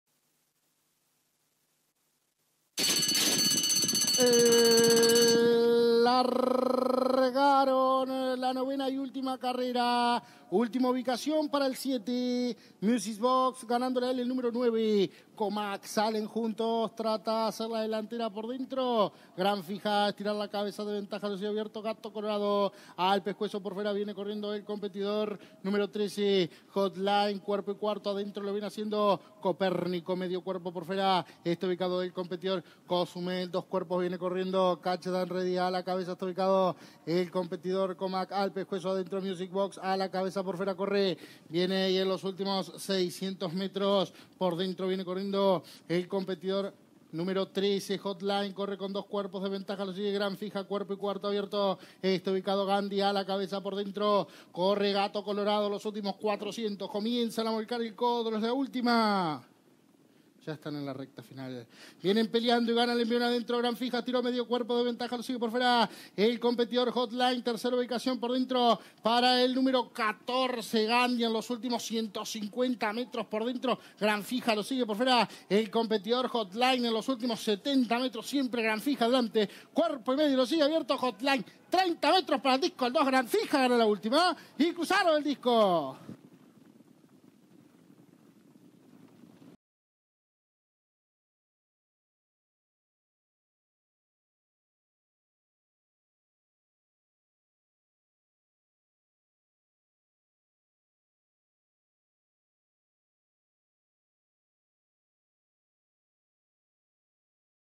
04/07/2015 – Hipodromo Las Piedras – Carrera 9 – JUAN C. ECHENIQUE -Cuidador-
Carrera 9 realizada el dia 04/07/2015 por el premio JUAN C. ECHENIQUE -Cuidador- en Hipodromo Las Piedras